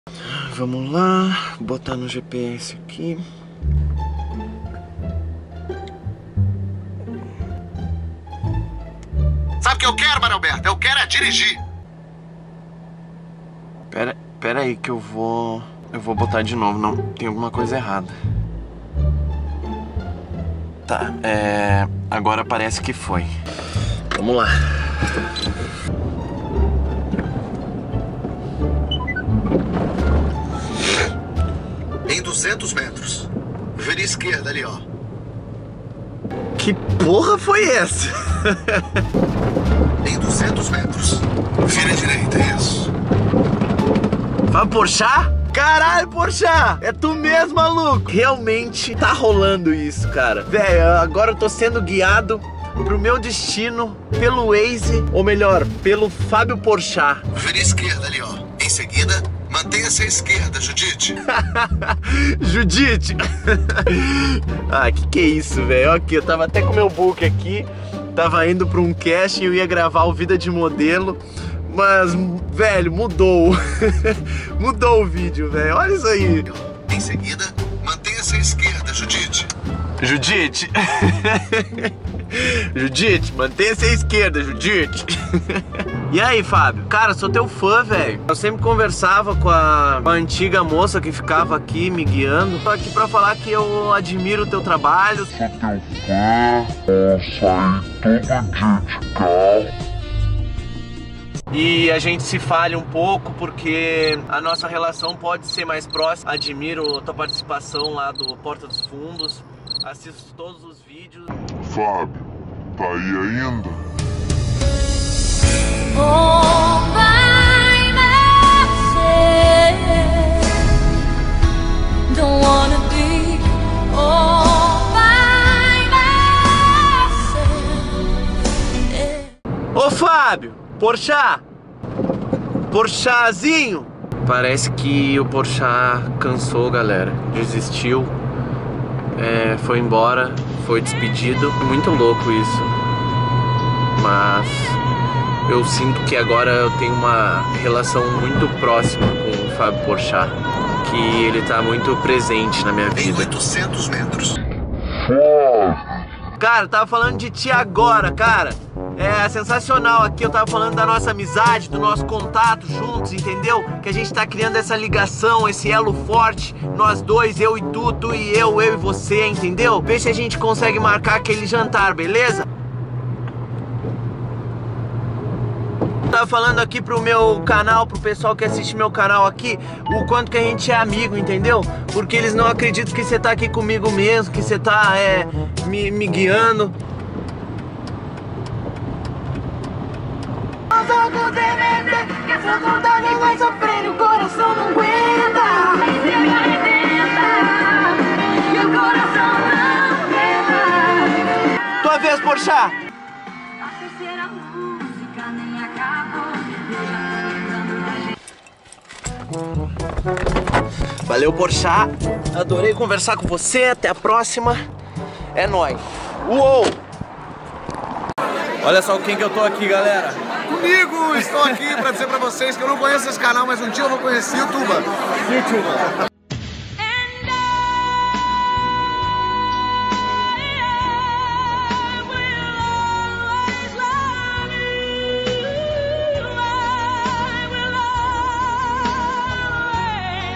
FÁBIO PORCHAT PEGOU CARONA COMIGO! FT. PORTA DOS FUNDOS
O DIA EM QUE EU CONHECI O FÁBIO PORCHAT(PORTA DOS FUNDOS), ELE PEGOU CARONA DE CARRO COMIGO E AINDA ME GUIOU PRO MEU DESTINO, NO FINAL DO VÍDEO ELE TEM UM RECADINHO PRA DAR PRA VOCÊS! ASSISTA ATÉ O FINAL!! ♥

"EM UM RELACIONAMENTO SÉRIO"

ºDEIXA O LIKE QUE ESSE FOI UM DOS VÍDEOS MAIS FODAS DO CANAL PRA MIM!! ♥
ºCOMPARTILHA?! (NUNCA TE PEDI NADA)

PODE PÁ QUE É NOIZ